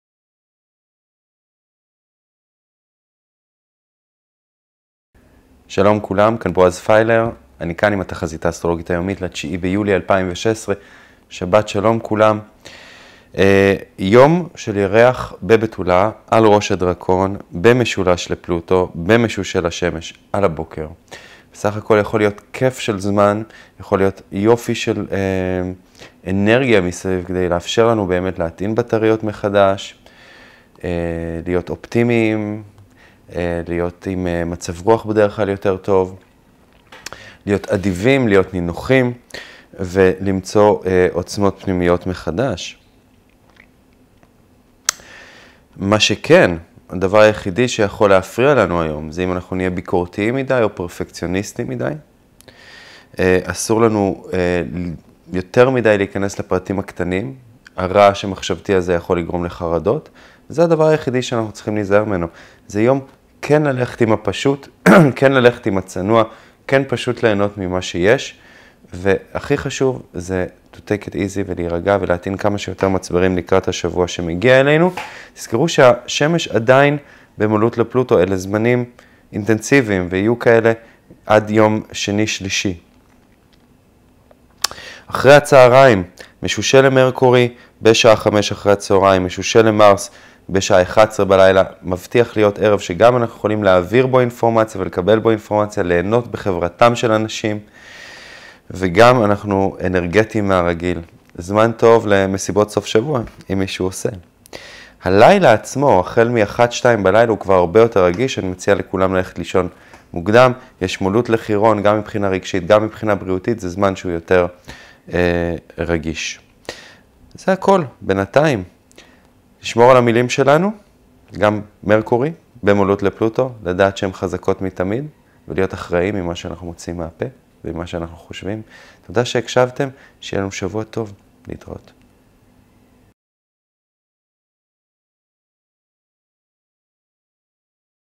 התחזית האסטרולוגית היומית עם בועז פיילר 09 יולי 2016
יכול להיות כיף של יום אם רק לא נהיה פרפקציוניסטים וביקורתיים מידי.